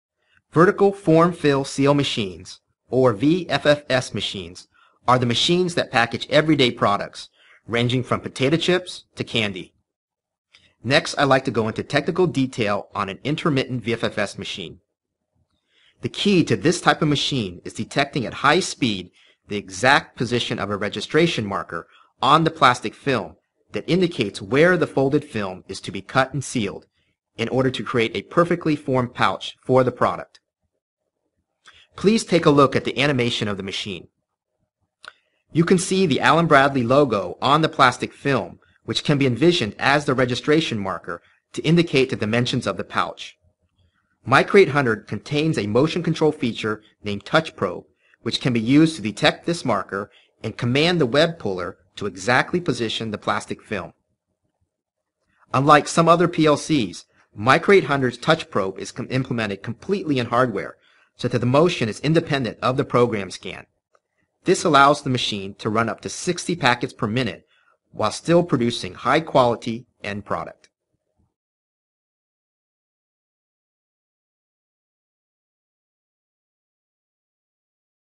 Connected Components Solution for Intermittent VFFS
The animation explains how Micro850 controller can be applied to industrial applications such as Intermittent VFFS machine.